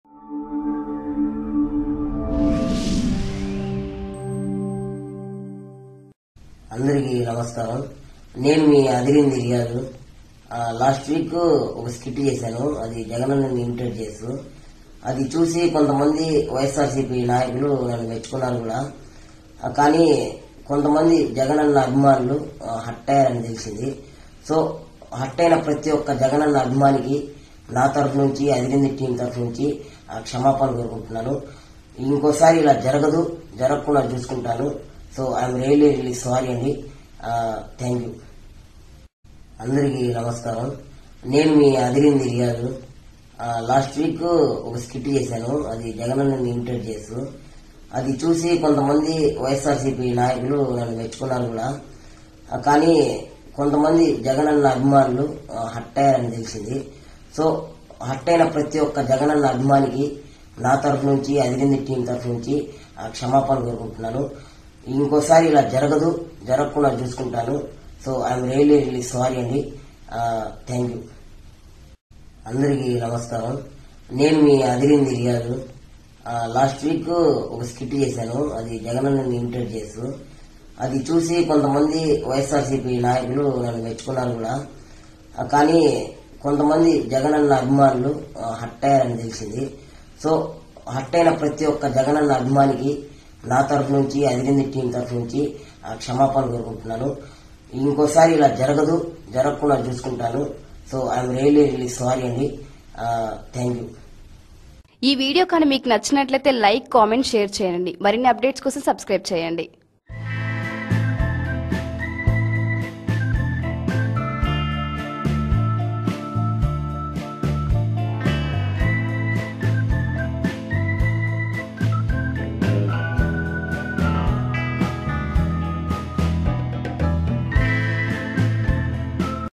Adirindi Comedian Says Sorry To AP CM YS Jagan | Adirindhi Comedian Imitates YS Jagan | Indiontvnews
Adirindi Comedian Says Sorry To AP CM YS Jagan. Adirindhi Comedian Imitates YS Jagan.